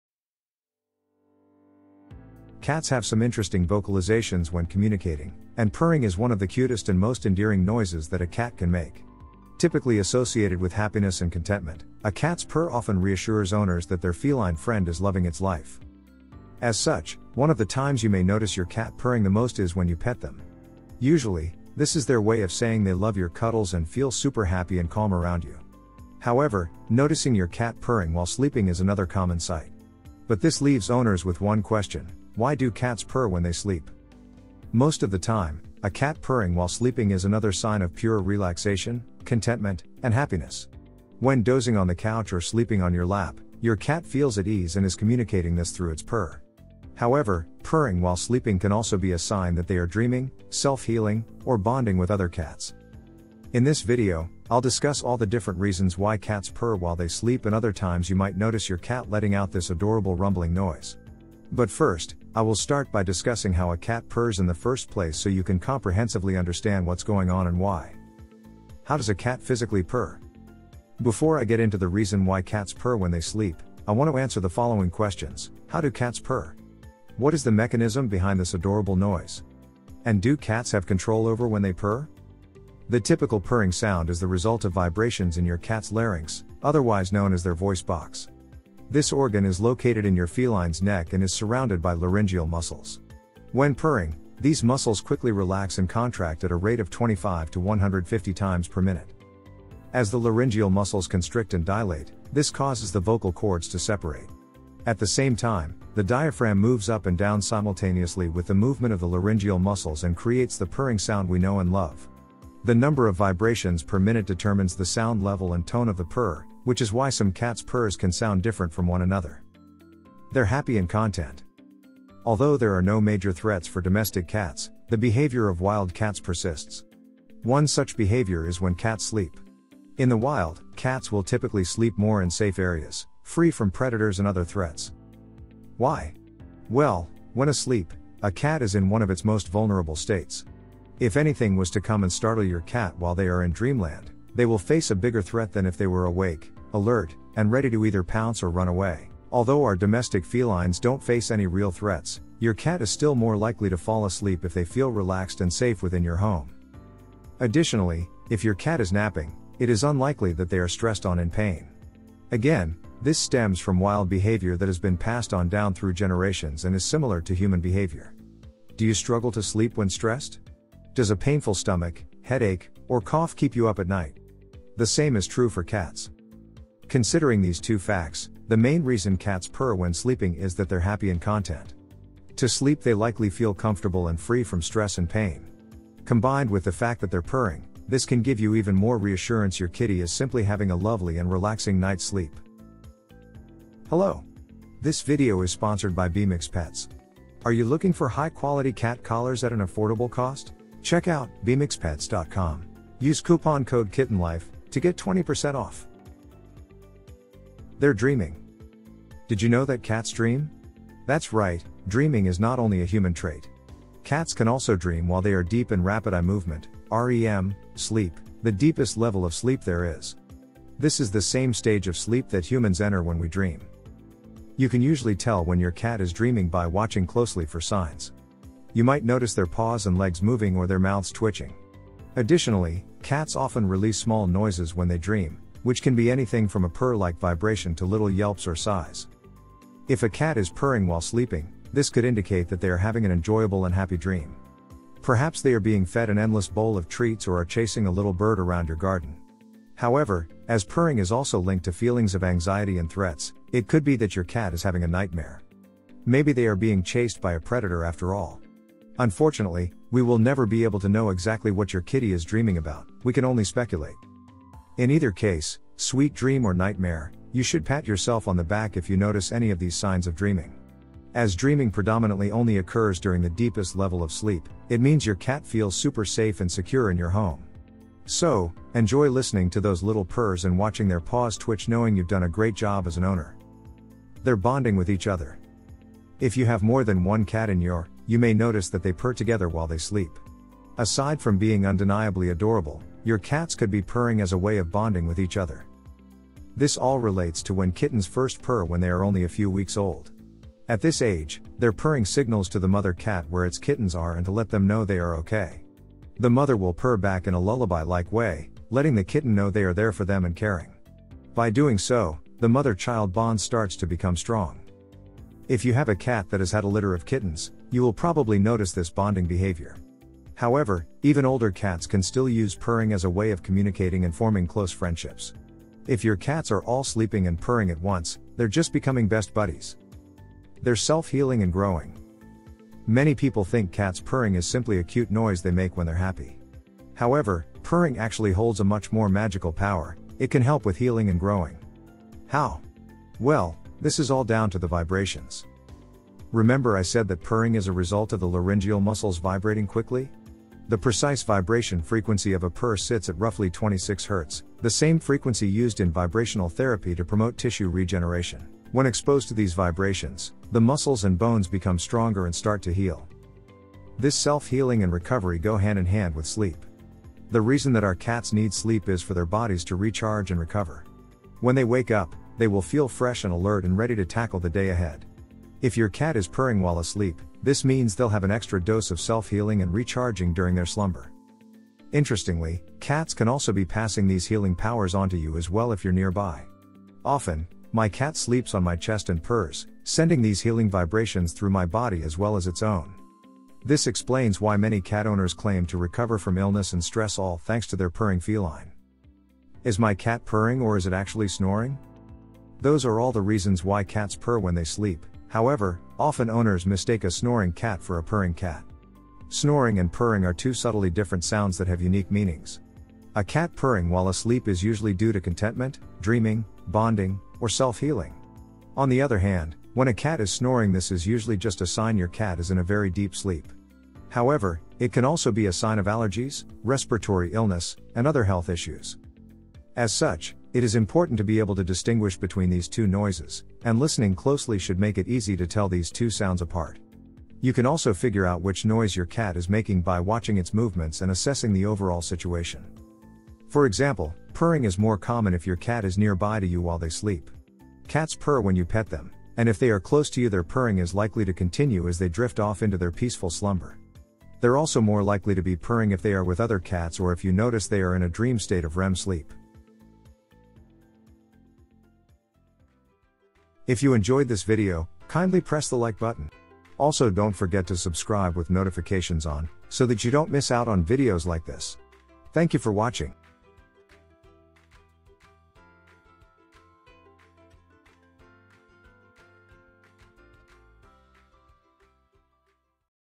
Why Do Cats Purr When They Sleep?
When talking, cats have some unusual vocalizations, and purring is one of the sweetest and most adorable noises a cat can produce. A cat's purr, which is typically connected with happiness and contentment, often reassures owners that their feline buddy is enjoying its existence.

As a result, when you pet your cat, you may notice them purring the most. This is usually their way of telling they like your embraces and feel quite pleased and relaxed with you. However, another usual sight is your cat purring while sleeping. However, this begs the issue of why cats purr when sleeping.